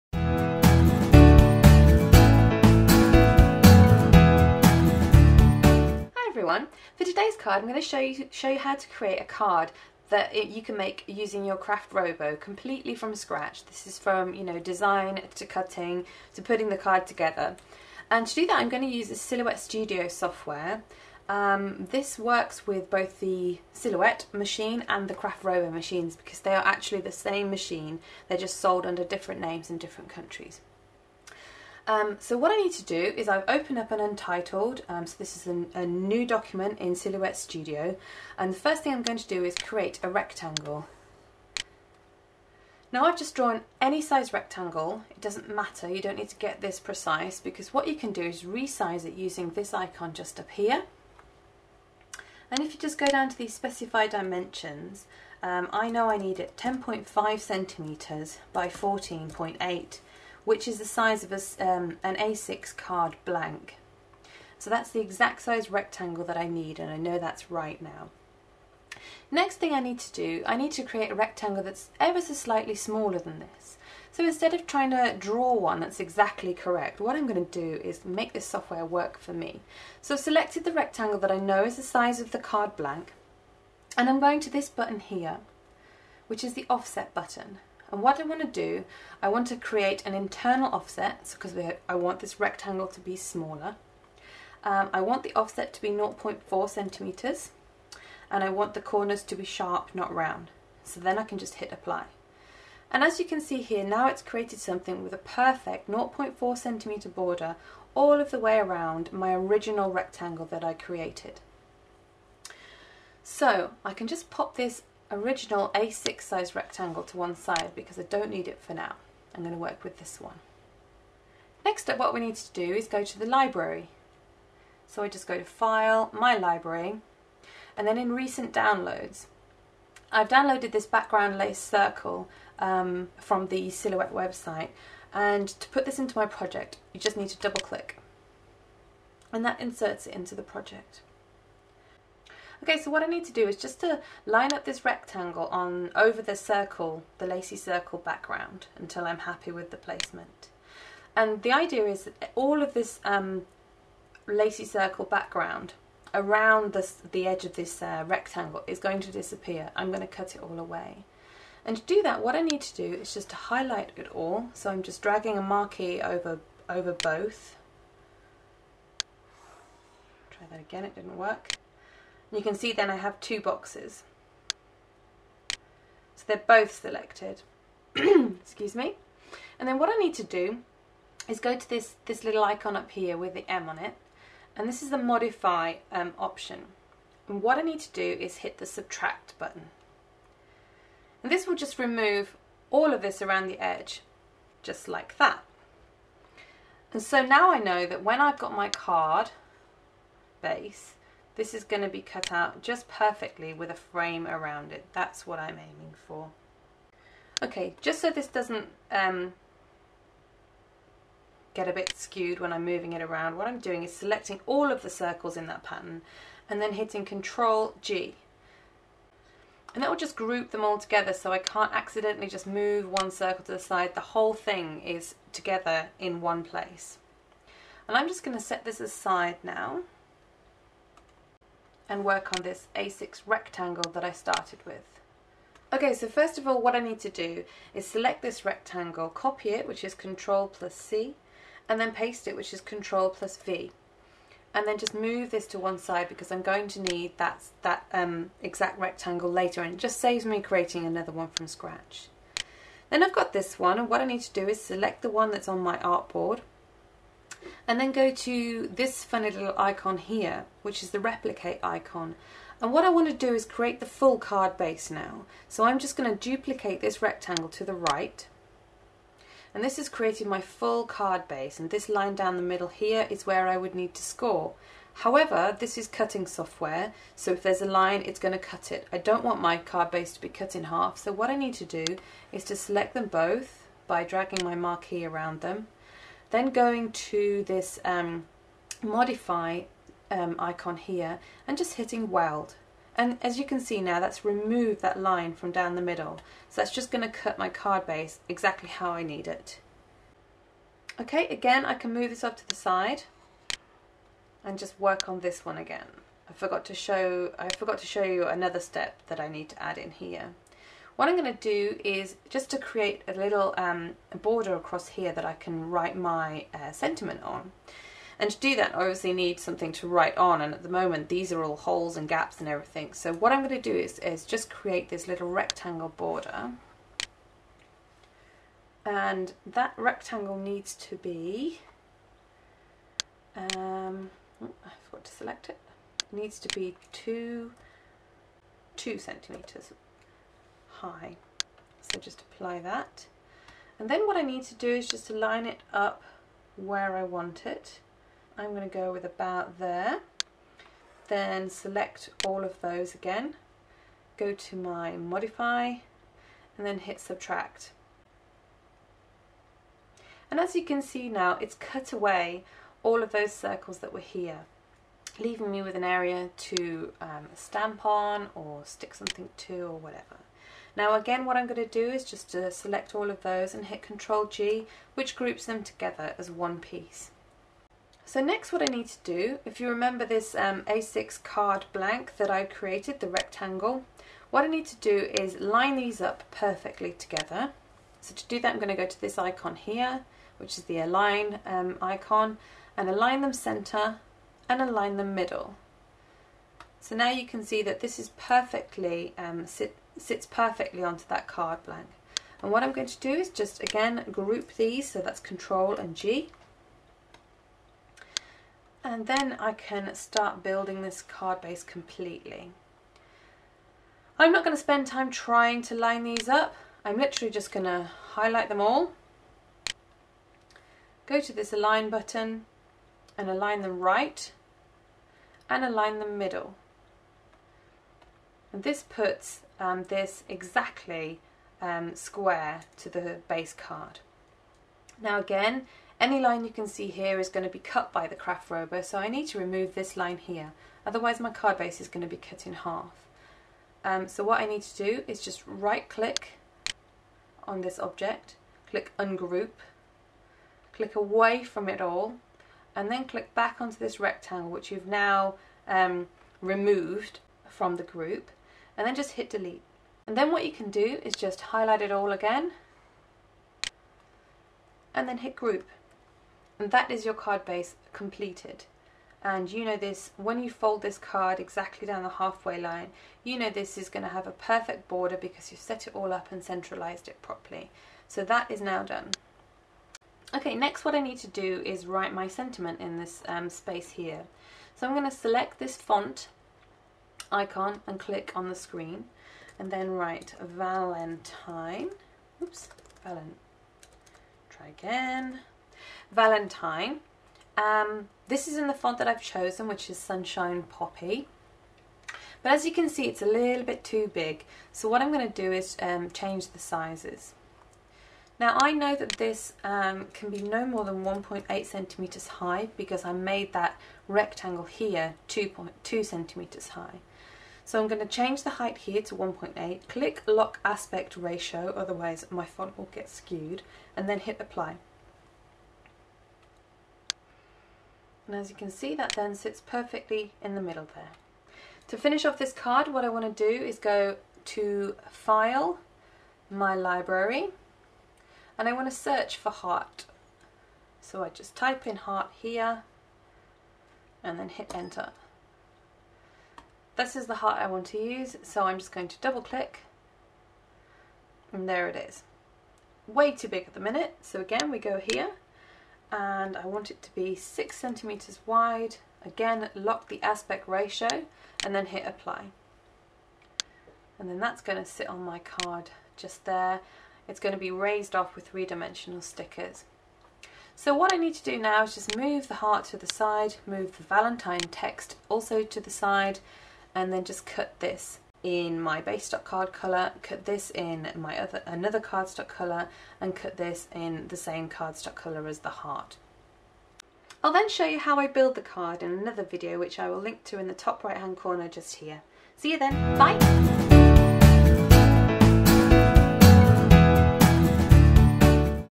Silhouette Studio Tutorial - Valentine Heart card (part 1 of 2)
Creating a die cut card design using the Silhouette Studio software. I make the card from all the die cuts in my next video

Happy crafting! =)